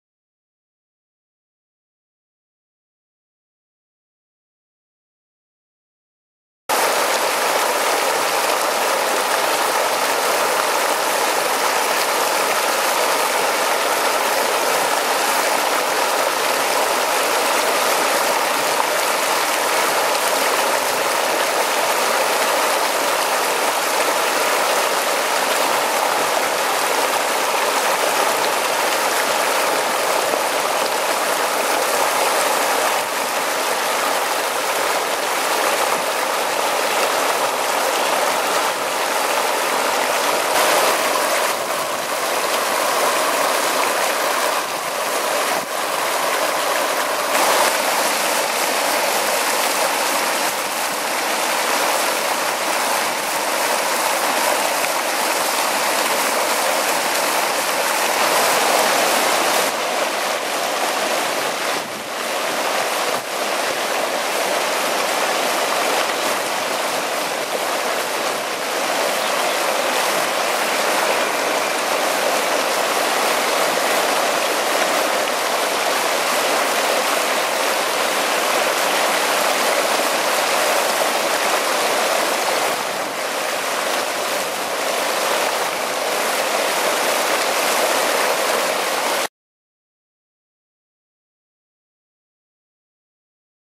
Streaming water. HD CC
A relaxing video of streaming water.
The sound of the water and the close up of the water is very relaxing.
We hope it gives you a moment to enjoy.

Under the license of Creative Commons.